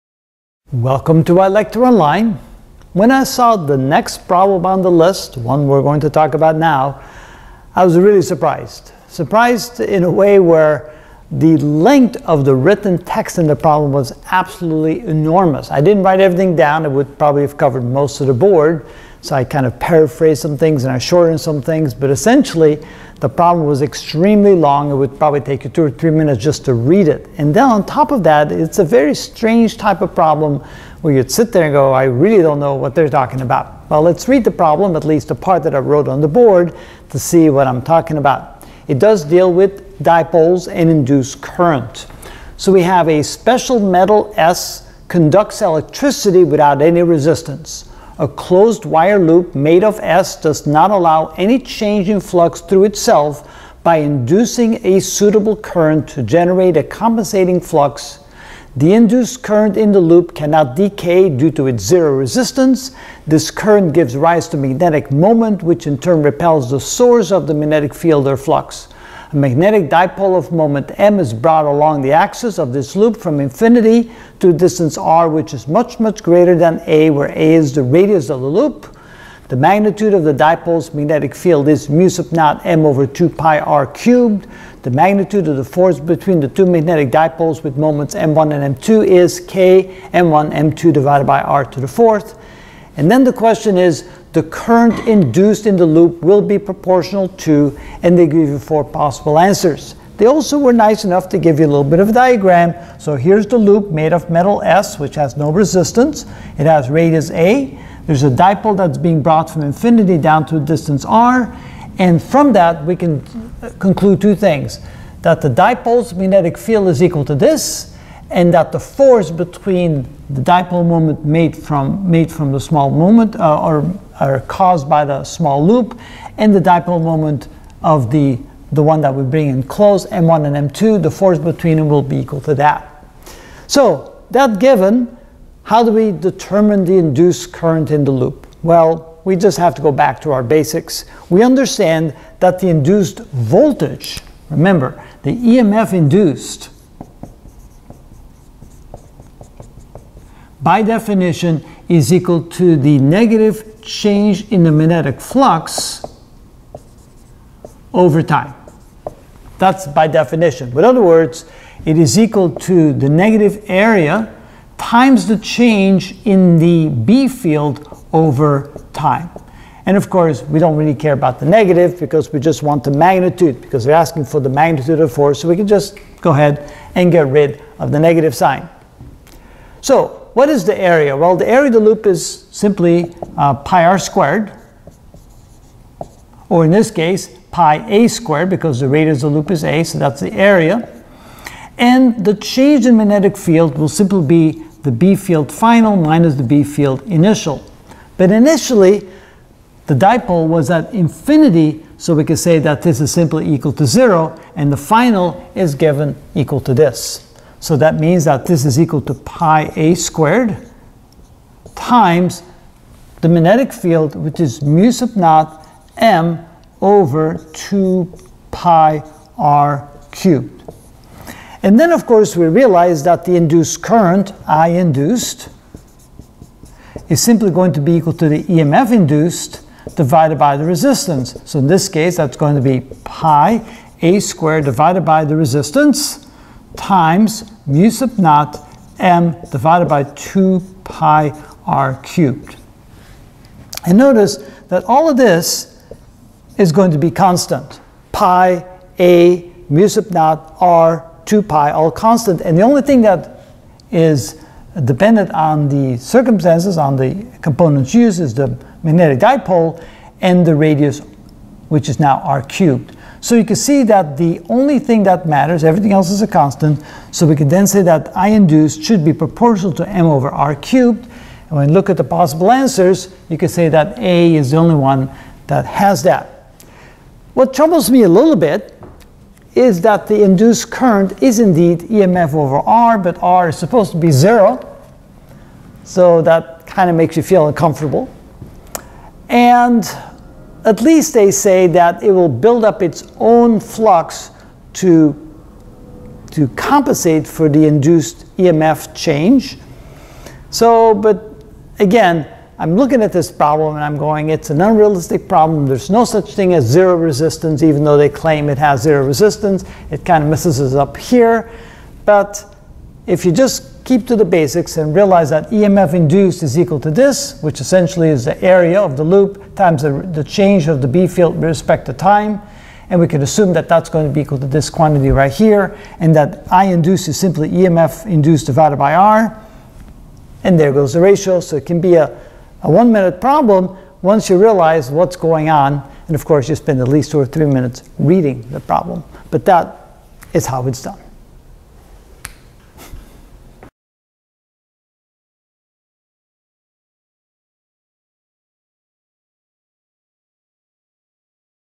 JEE Advanced Physics 2021 Paper 2 #13 Dipole and Induced Current (Part 1)
(Part 1) A special metal S conducts electricity without any resistance. A closed wire loop, made of S, does not allow any change in flux through itself by inducing a suitable current to generate a compensating flux. The induced current in the loop cannot decay due to its zero resistance. This current gives rise to a magnetic moment which in turn reples the source of the magnetic field or flux. A magnetic dipole of moment m is brought along the axis of this loop from infinity to r is much, much less than a. The magnitude of the dipole’s magnetic field is (u0)(m)/[2(pi)r^3]. The magnitude of the force between the two magnetic dipoles with moments m1 and m2 is (k)(m1)(m2)/(r^4). The current induced in the loop will be proportional to:
A) m/r^3
B) m^2/r^2
C) m/r^2
D) m^3/r